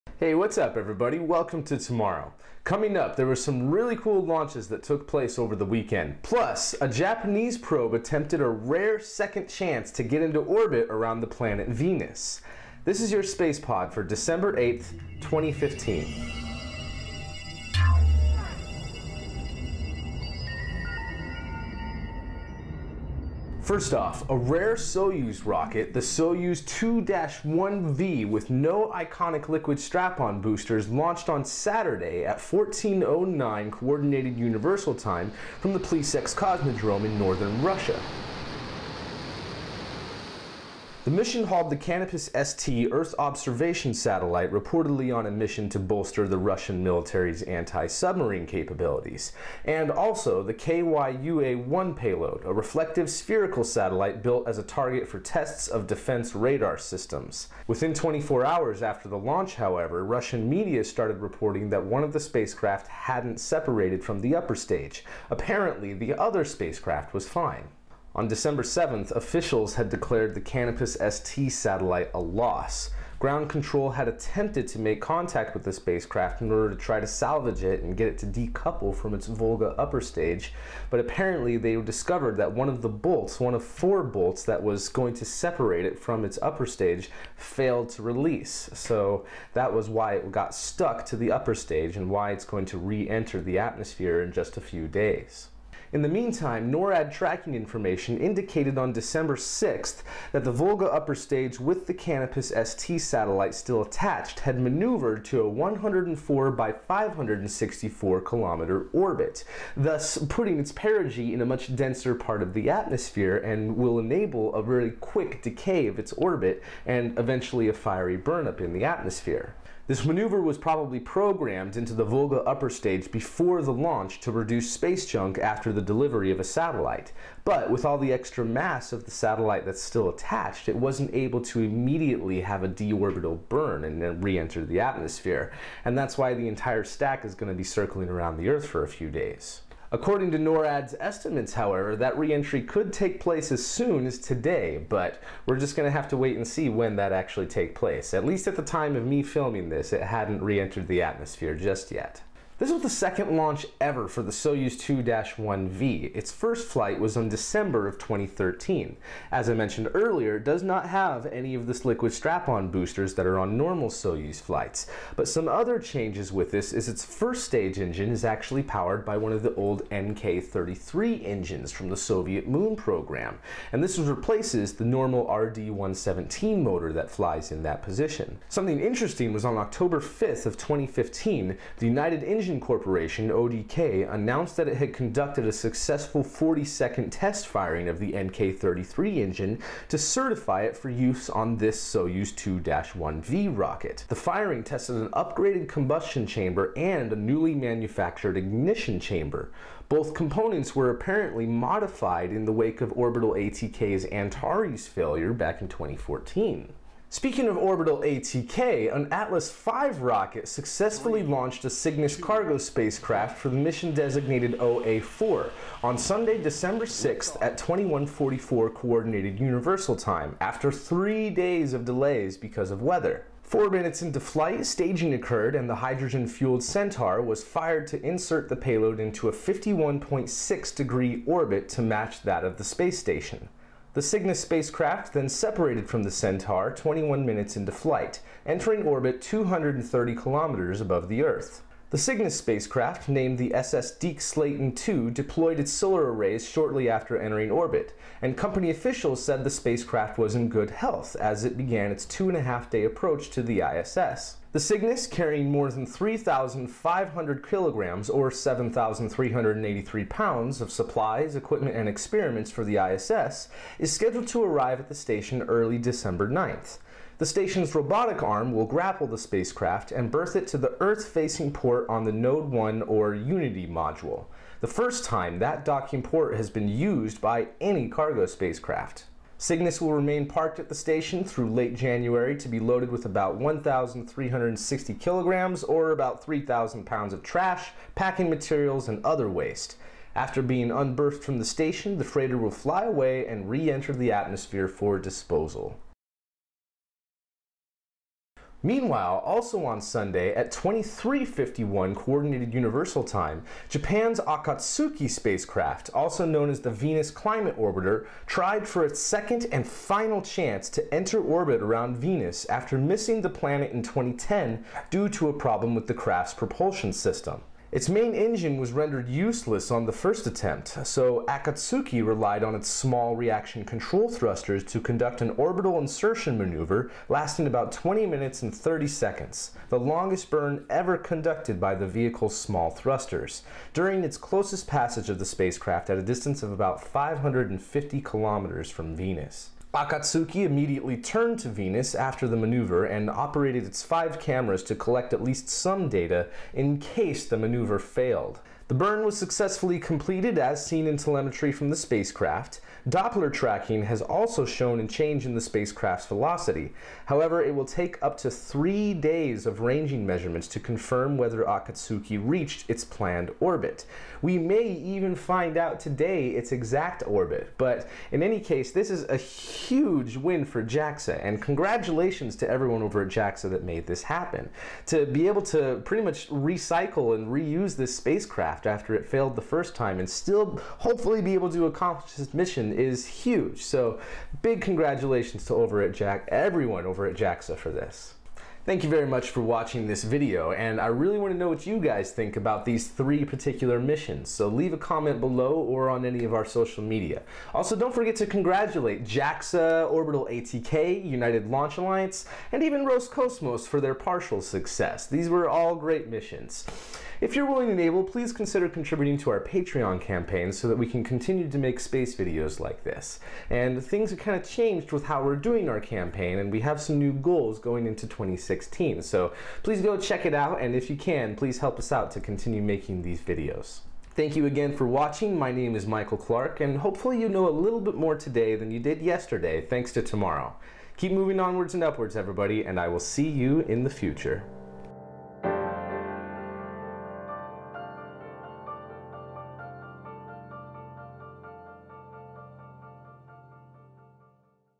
Akutsuki Orbits Venus and Cygnus Launches - SpacePod 12/08/15
Several cool launches took place over the weekend and the Japanese Probe Akatsuki entered orbit around Venus.